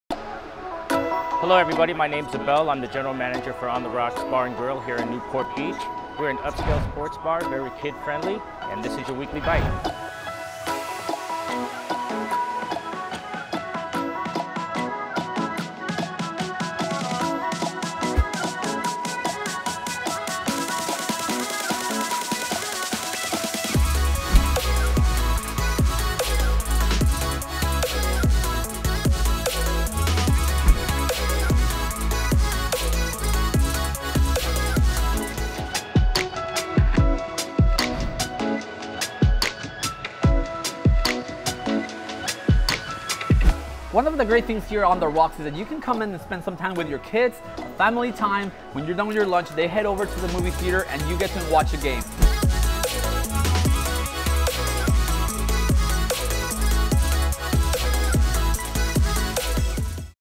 Weekly Bites: On the Rocks Bar & Grill
On the Rocks Bar & Grill is an All-American sports bar & grill with a great family atmosphere. They serve a wonderful array of California cuisine and take pride in delivering the best tasting foods to please the most eclectic palette.